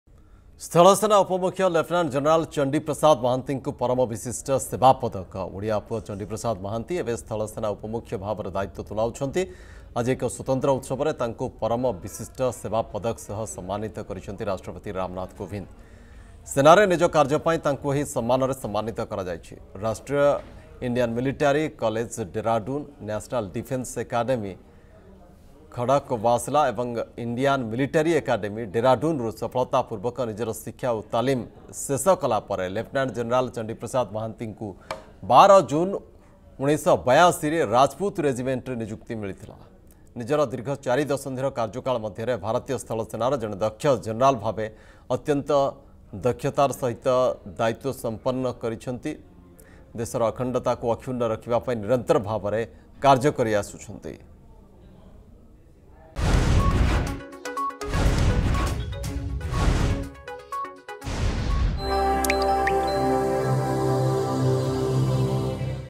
ସ୍ଥଳସେନା ଉପମୁଖ୍ୟ ଲେଫ୍ଟନାଣ୍ଟ ଜେନେରାଲ ଚଣ୍ଡି ପ୍ରସାଦ ମହାନ୍ତିଙ୍କୁ ପରମ ବିଶିଷ୍ଟ ସେବା ପଦକ
ସ୍ଥଳସେନା ଉପମୁଖ୍ୟ ଲେଫ୍ଟନାଣ୍ଟ ଜେନେରାଲ ଚଣ୍ଡି ପ୍ରସାଦ ମହାନ୍ତିଙ୍କୁ ପରମ ବିଶିଷ୍ଟ ସେବା ପଦକ। ଓଡ଼ିଆ ପୁଅ ଚଣ୍ଡିପ୍ରସାଦ ମହାନ୍ତି ଏବେ ସ୍ଥଳ ସେନା ଉପମୁଖ୍ୟ ଭାବେ ଦାୟିତ୍ବ ତୁଲାଉଛନ୍ତି । ଆଜି ଏକ ସ୍ୱତନ୍ତ୍ର ଉତ୍ସବରେ ତାଙ୍କୁ ପରମ ବିଶିଷ୍ଟ ସେବା ପଦକ ସହ ସମ୍ମାନିତ କରିଛନ୍ତି ରାଷ୍ଟ୍ରପତି ରାମନାଥ କୋବିନ୍ଦ